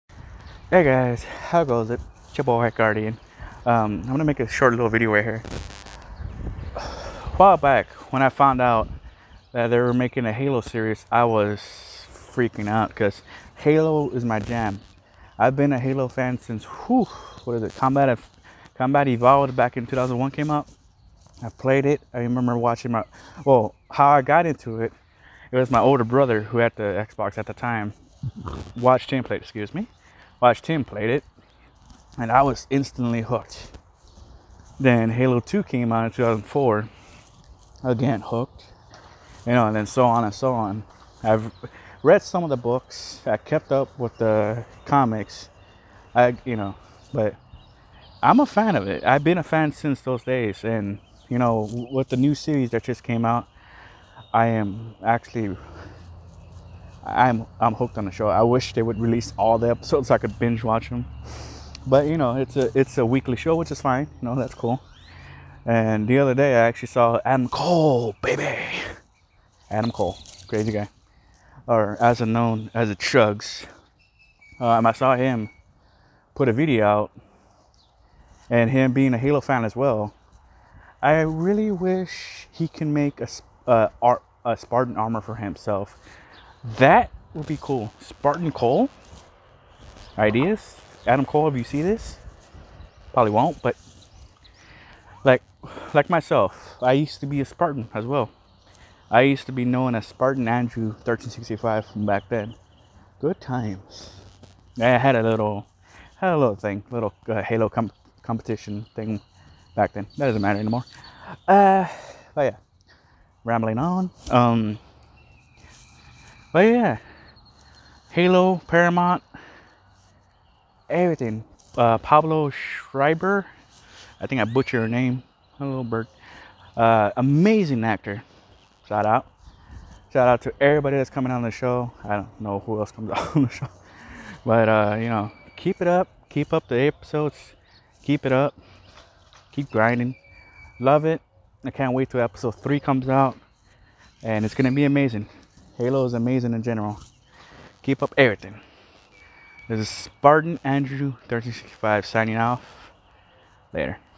Talking Abit About The New Halo Series!
short video talking about the halo series on paramount plus, Adam Cole Bay Bay aka theCHUGS, and Pablo Schreiber, who plays Master Cheif in the show. hope you guys check out Adam Cole on Twitch and enjoy the show!